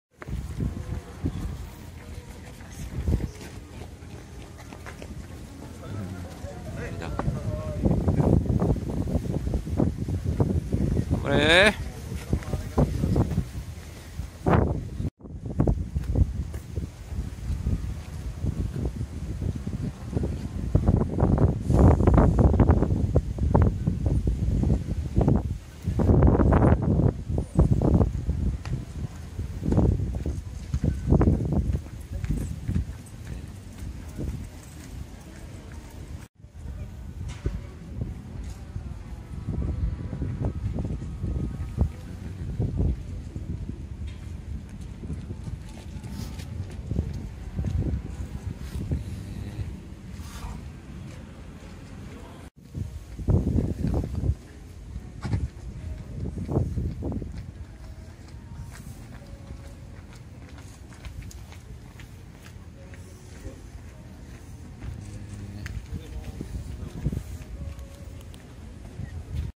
日本ハムファイターズ新人合同自主トレ 球場入2019/1/21
ドラフト1位 吉田輝星(金足農業高)
ドラフト2位 野村佑希(花咲徳栄高）
ドラフト3位 生田目翼(日本通運)
ドラフト4位 万波中正(横浜高)
ドラフト5位 柿木蓮(大阪桐蔭高)
ドラフト6位 田宮裕涼(成田高)
ドラフト7位 福田俊(星槎道都大学)
育成ドラフト1位 海老原一佳(富山GRNサンダーバーズ)